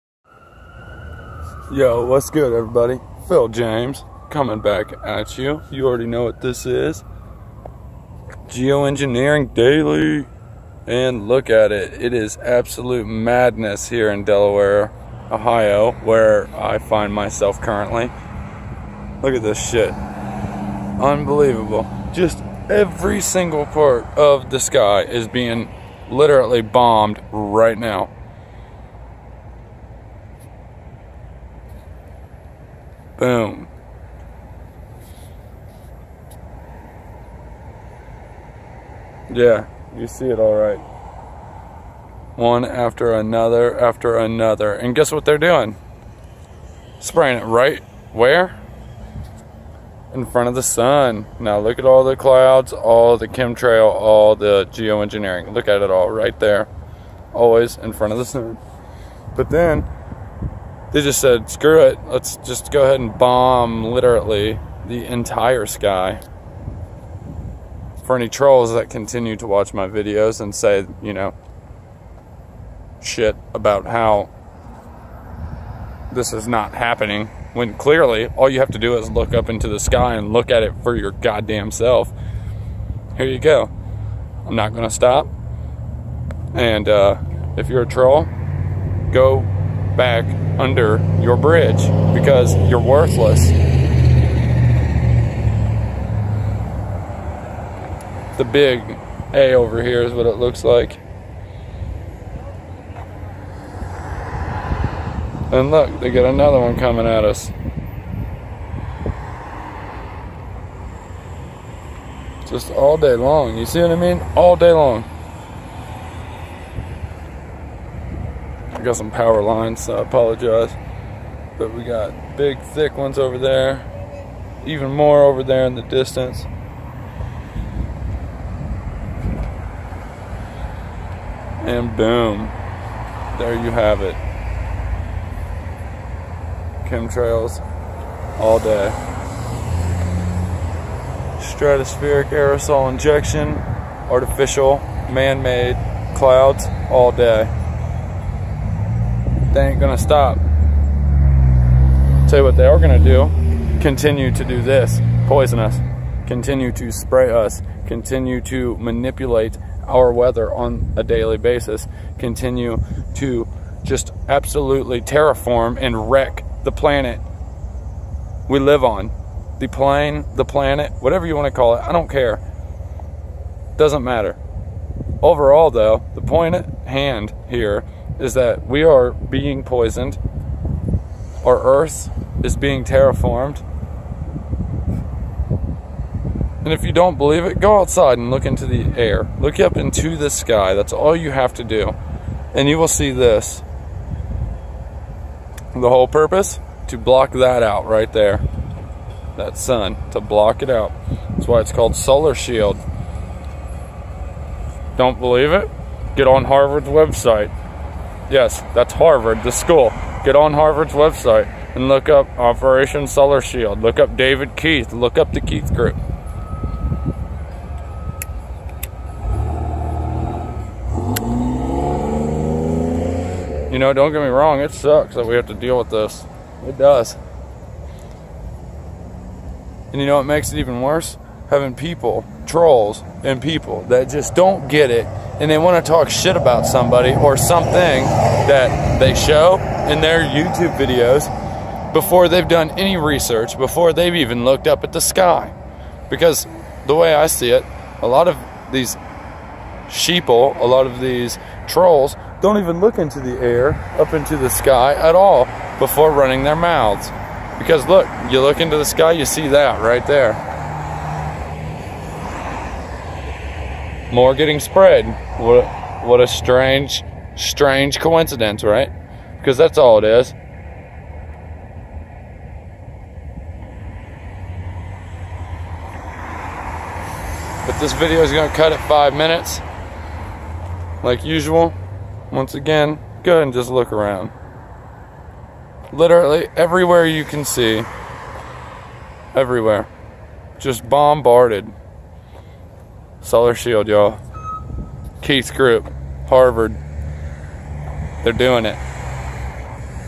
Geoengineering Daily #12
Delaware, Ohio is being absolutely bombarded by chemtrails right now. Its October 2, 2017. And, they are not going to stop... Trolls, controlled opposition, disinfo agents, suck it. You are a disgrace to humanity.